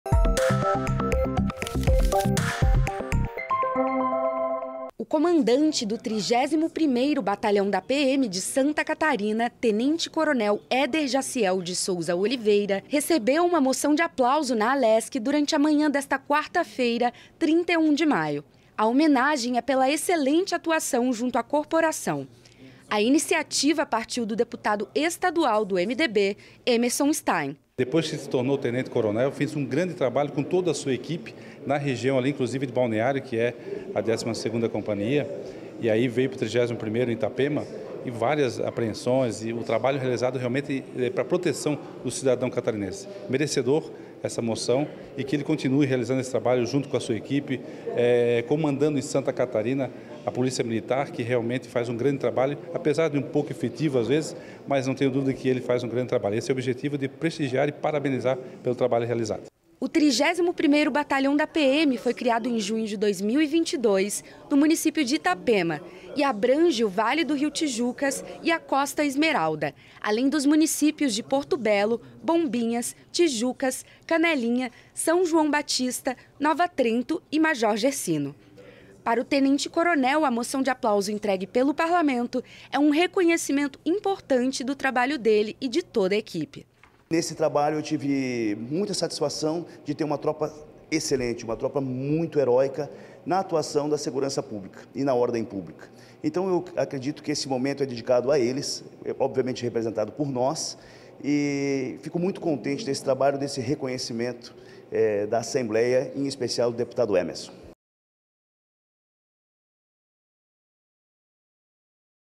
É Notícia - Comandante do 31º Batalhão da PM de Santa Catarina recebe Moção de Aplauso - 31/06/23
Por iniciativa do deputado estadual Emerson Stein (MDB), o Comandante do 31º Batalhão da PM de Santa Catarina, Ten. Cel. Eder Jaciel de Souza Oliveira, recebeu uma Moção de Aplauso na Alesc, durante a manhã desta quarta-feira (31/05). A homenagem é pela excelente atuação junto à corporação. O 31º Batalhão da PM foi criado em junho de 2022, no município de Itapema e abrange o Vale do Rio Tijucas e a Costa Esmeralda, além dos municípios de Porto Belo, Bombinhas, Tijucas, Canelinha, São João Batista, Nova Trento e Major Gercino.